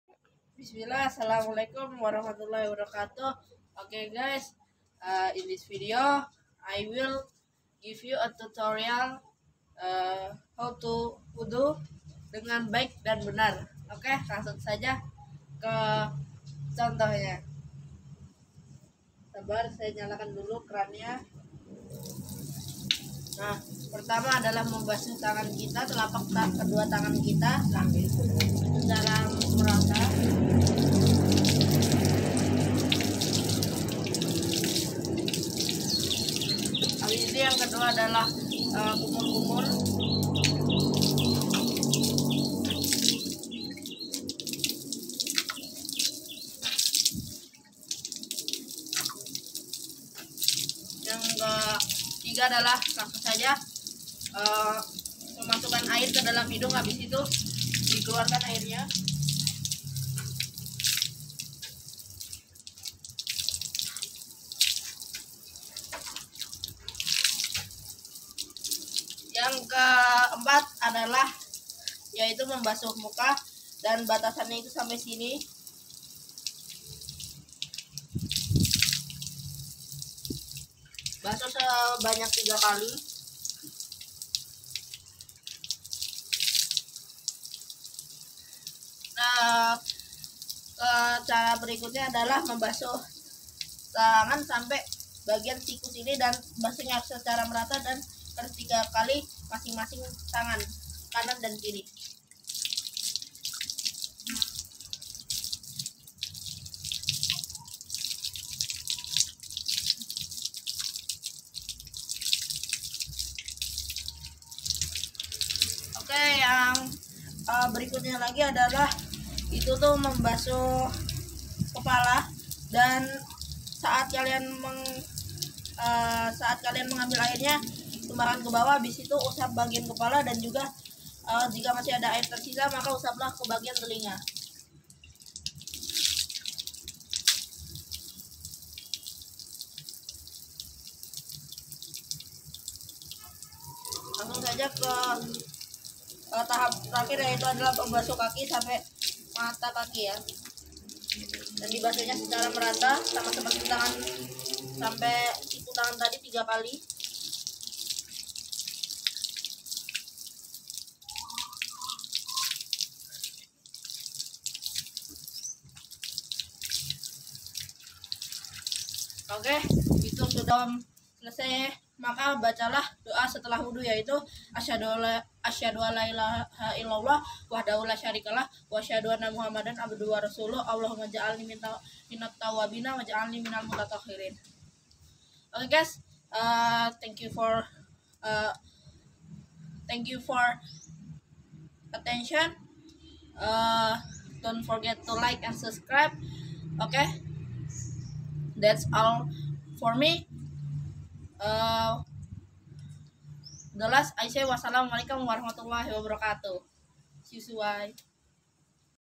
Cara wudhu dengan benar dan sesuai urutan nya
Disini saya punya vidio diniyah yaitu tutorial wudhu dengan benar dan sesuai urutan nya